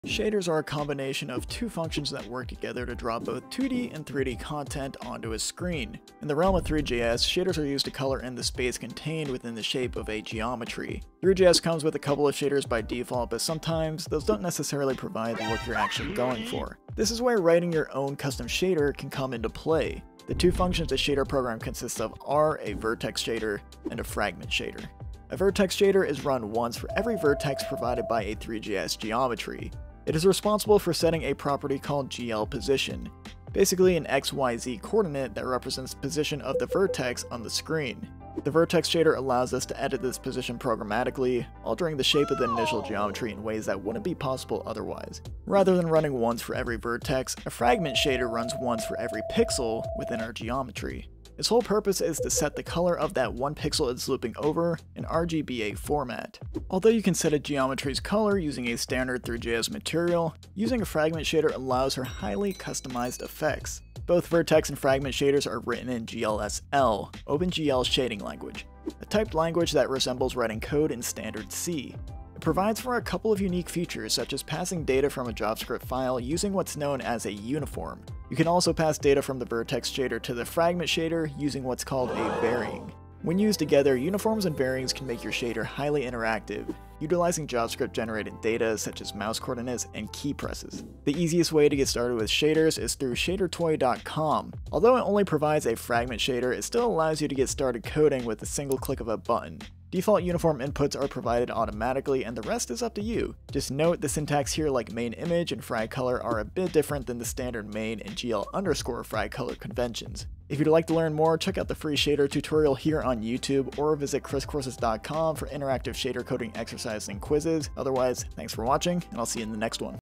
Three.js Shaders in 2 Minutes
Shaders utilize the GPU to render 2D and 3D objects onto a screen. With two simple functions: a vertex shader and a fragment shader, you can alter meshes programmatically to create never-before-seen visuals and effects.

There's a lot of jargon within this realm of development, so this video should help out a bit.